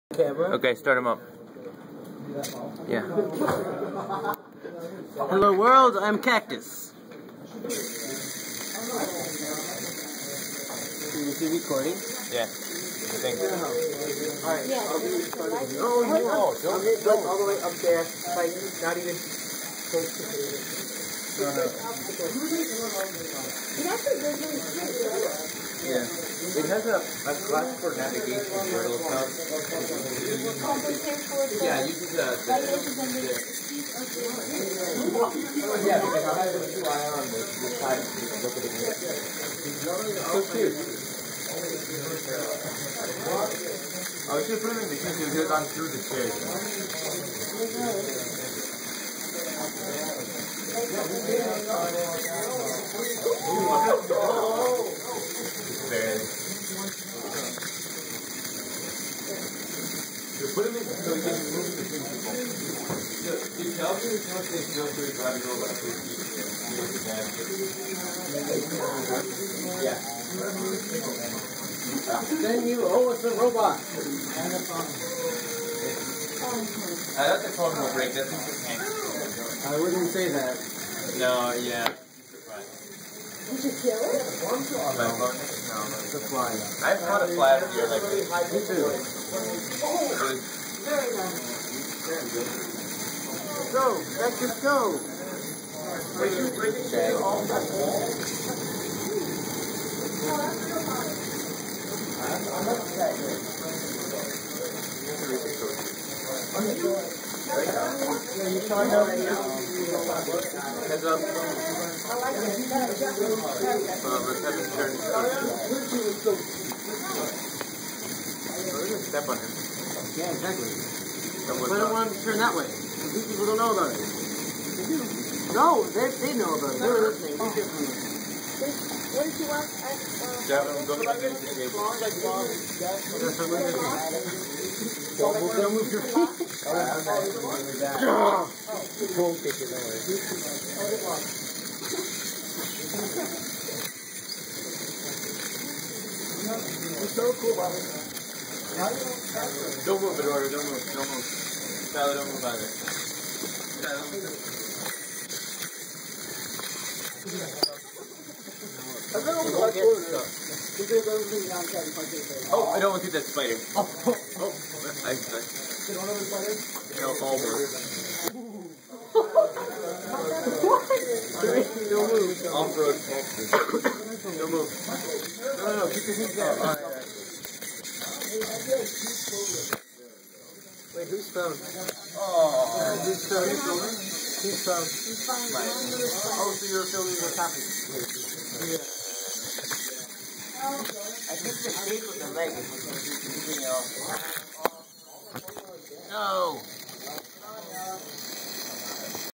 Cactus Nav [Test 1]
Test of the front facing IR sensors used for nav.
Cactus roams around the room and will try to avoid any problems it encounters